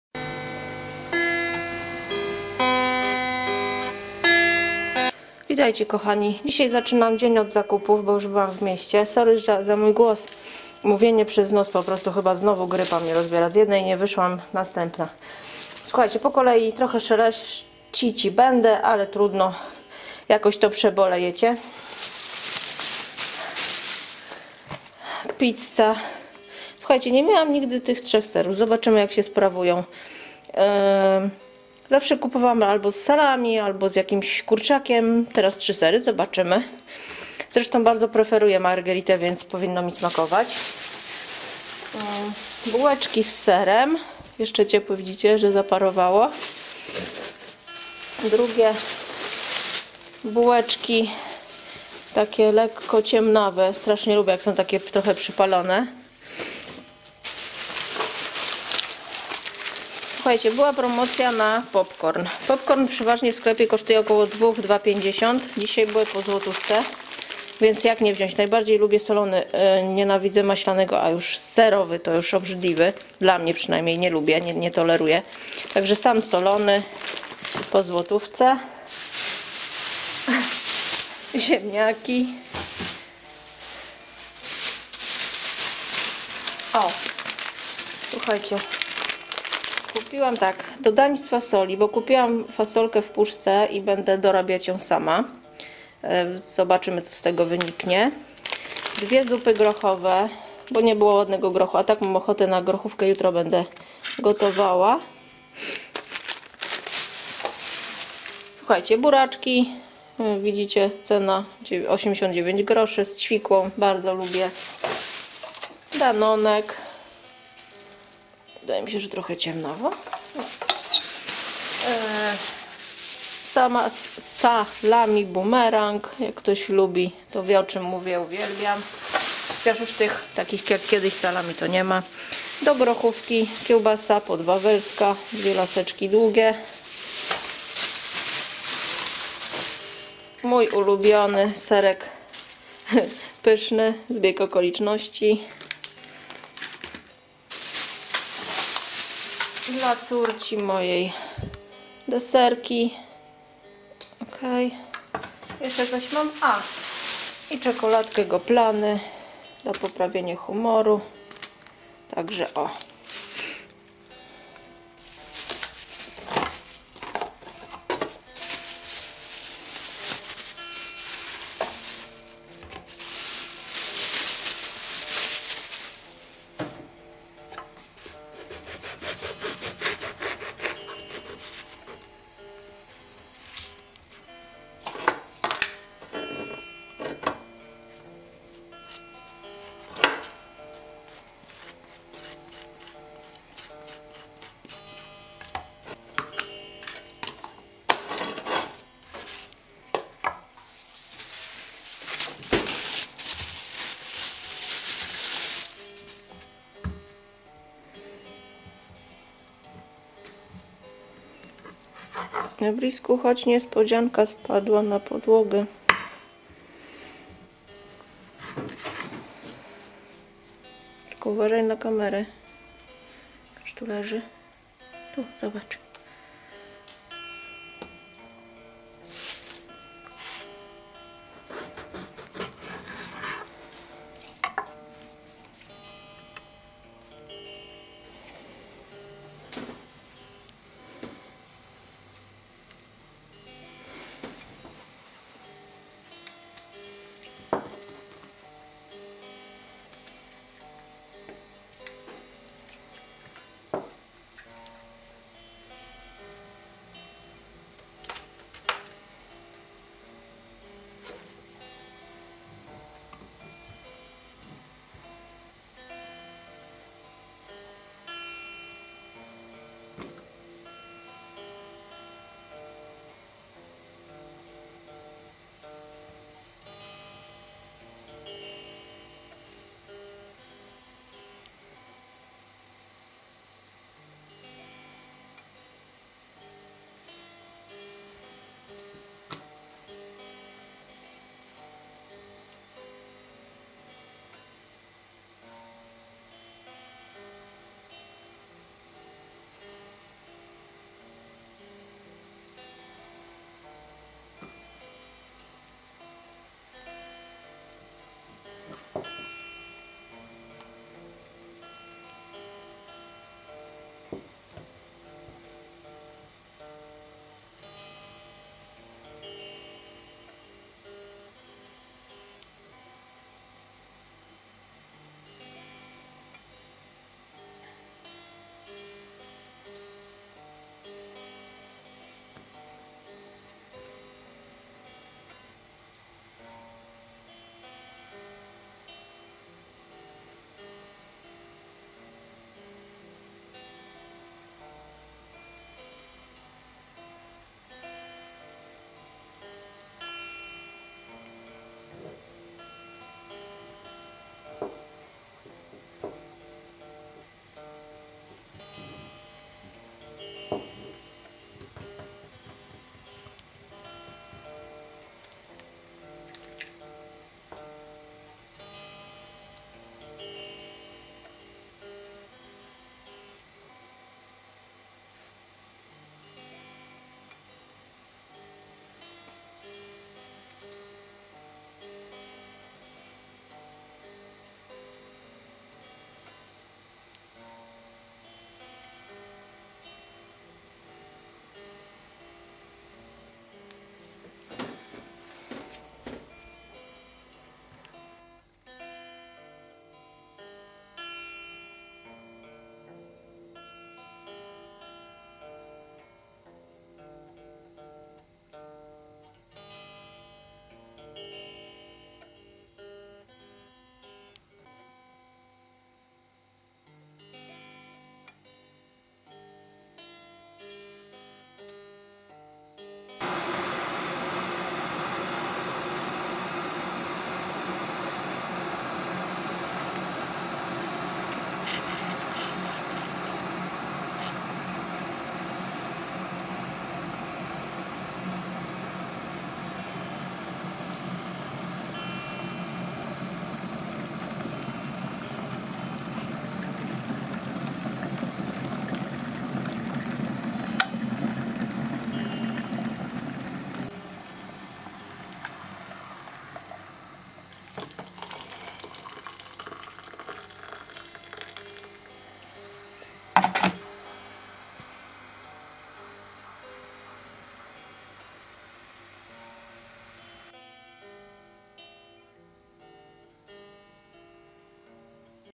165=Gulasz na sojowym,razowa z roladą i metamorfoza Moni czyli moja.
:) Lekki chochlik mi się wdarł pomiędzy filmik;)

MSK.

Sprzęt, którym nagrywam, to:
CANON 550D…

Jeśli tylko masz na to ochotę….wysyłaj tutaj...:
Monika JEZIERSKA